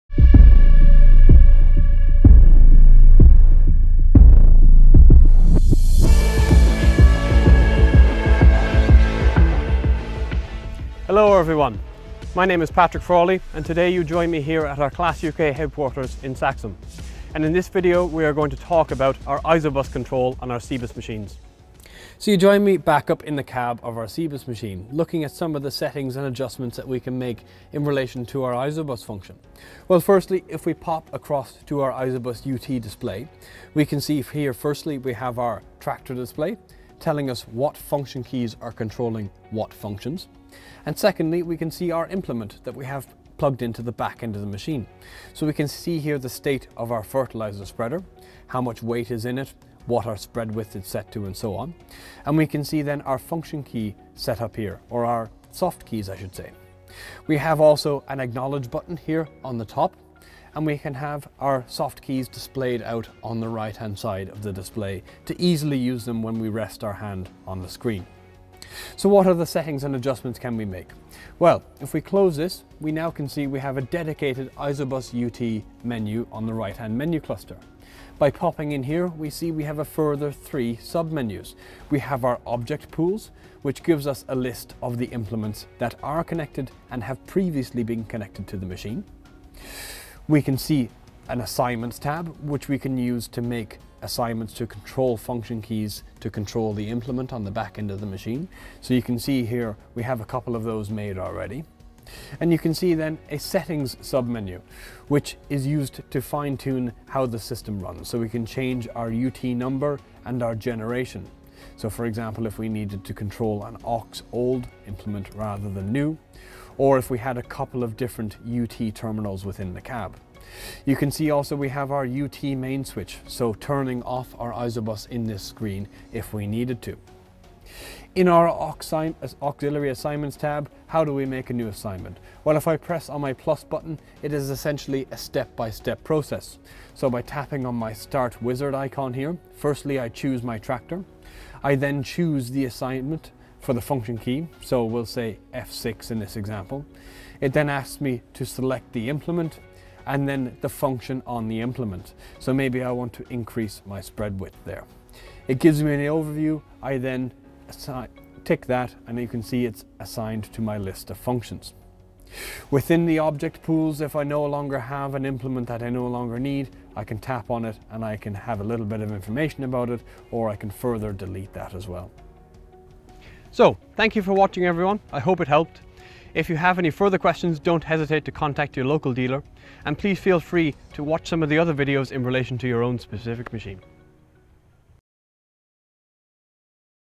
ISOBUS | CEBIS CLAAS Tractor
Here in this video the ISOBUS function on the CEBIS screen is explained, how best to use the feature and how to adjust the settings in relation to it. This applies to ARION 500, 600 with CEBIS and AXION 800, 900 with CEBIS.